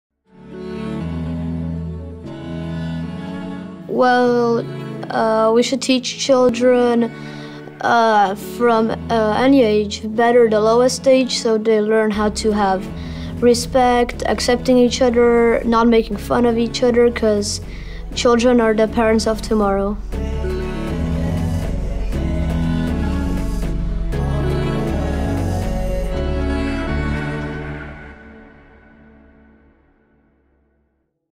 Youth Inclusion in Peacebuilding (Teaser 4)
Over 40% of the world's population is under the age of 25. This brief teaser highlights the importance of including the voices of young people in peacebuilding.